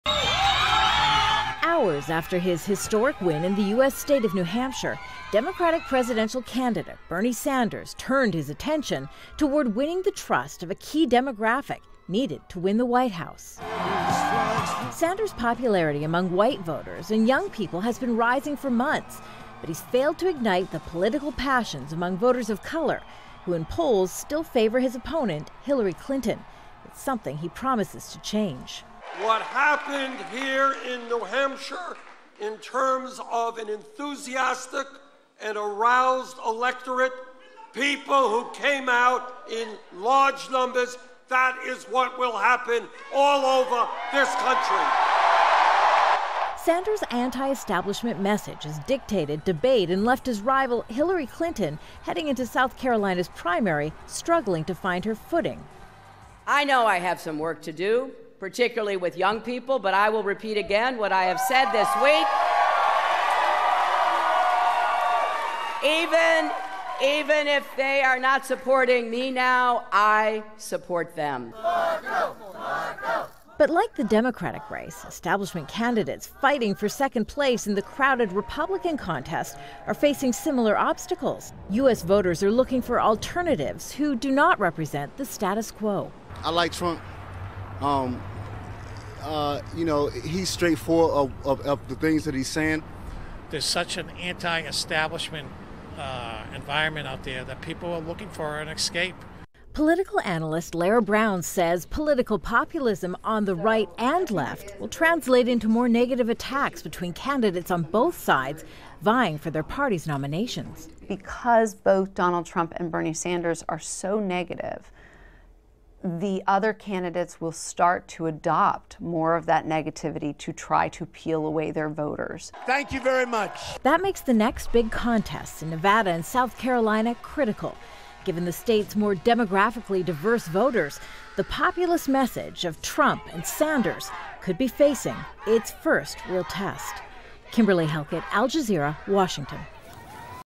US election: Candidates eye next contest
The New Hampshire primary saw big victories for two outsiders in the US presidential race.

Al Jazeera's Kimberly Halkett has more on the next steps in the race for the White House.